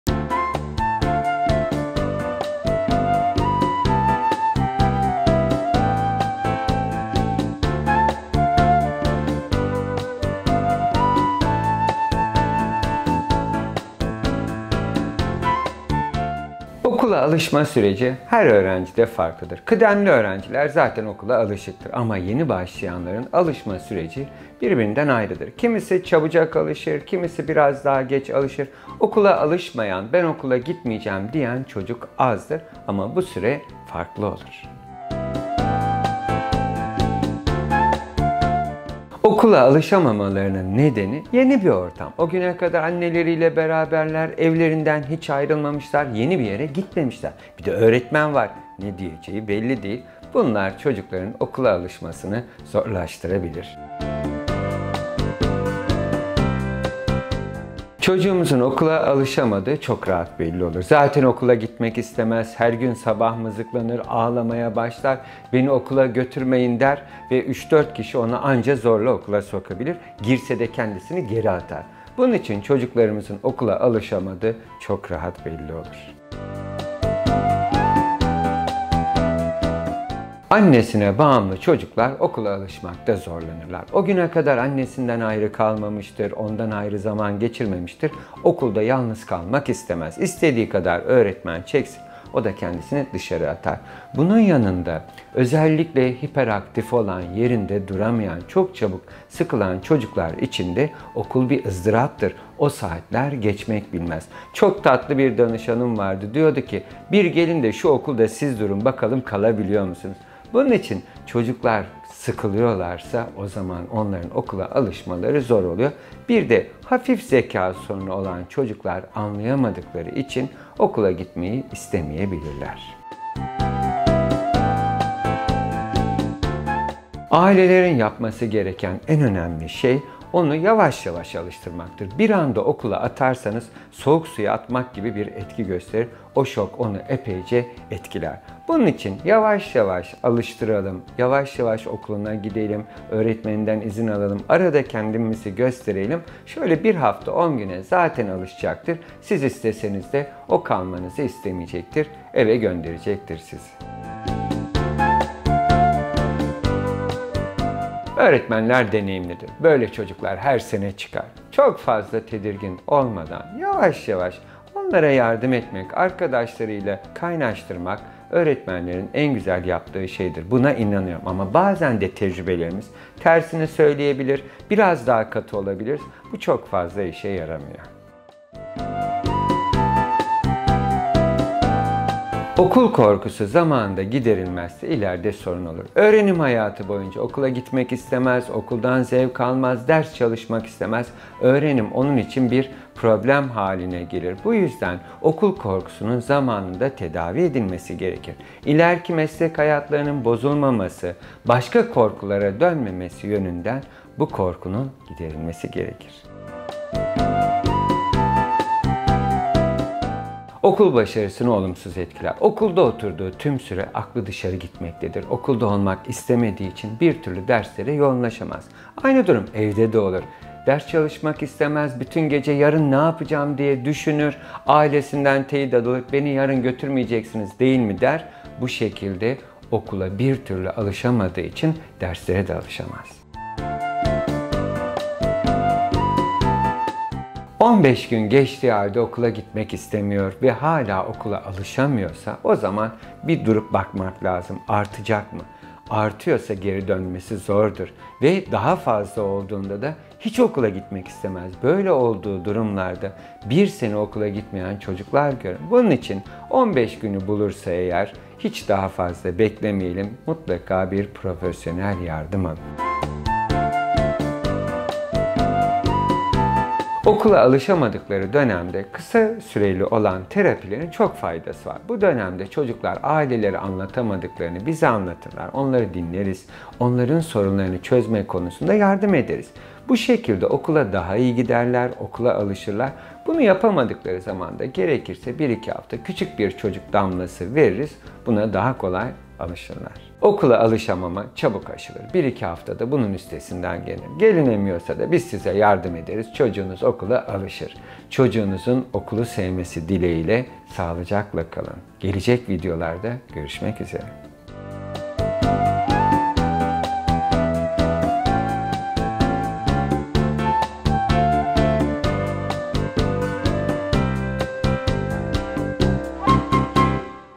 10 ADIMDA OKULA ALIŞTIRMA SÜRECİ
Psikiyatrist Dr. Sabri Yurdakul Okula alıştırma sürecini anlatıyor.

Kurtuluş Mah. 64019 Sk. No:32 Pakyürek İş Merkezi K:1 D:2 01030 Seyhan, Adana 01030 Seyhan, Adana